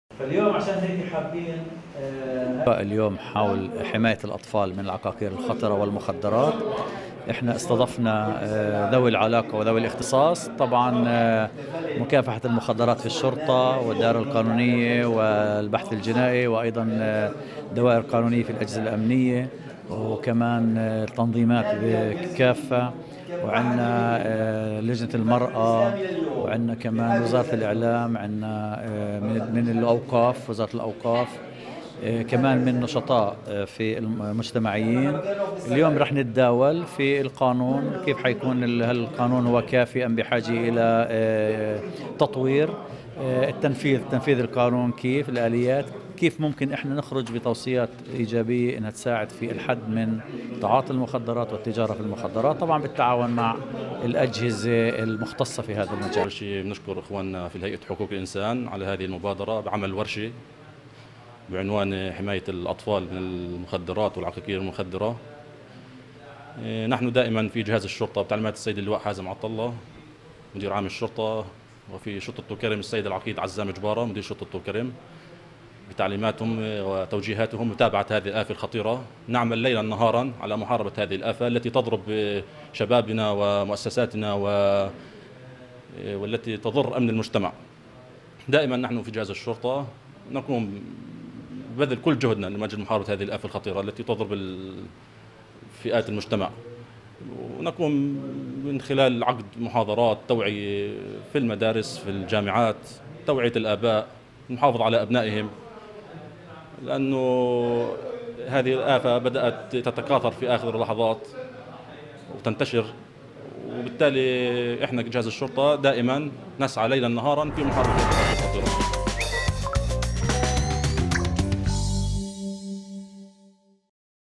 الهيئة المستقلة لحقوق الإنسان تنظم ورشة عمل حول ظاهرة المخدرات وكيفية الحد منها
تلفزيون الفجر 1/11/2018 | الهيئة المستقلة لحقوق الإنسان تنظم ورشة عمل حول ظاهرة المخدرات وكيفية الحد منها